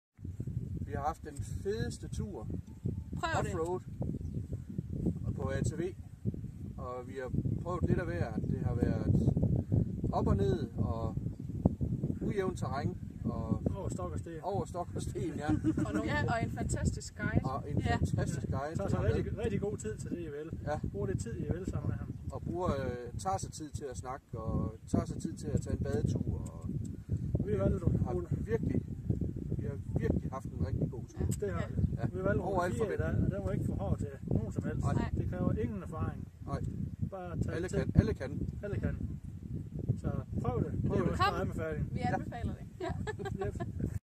Danish Tourists speak for Compass Outdoor Adventures
ATVs (QUADs) Route 4.. Sivota Thesprotia...
Holidays is not only beach and swimming,,,, is also mountain and adventures...
COMPASS OUTDOOR ADVENTURES - Sivota Thesprotia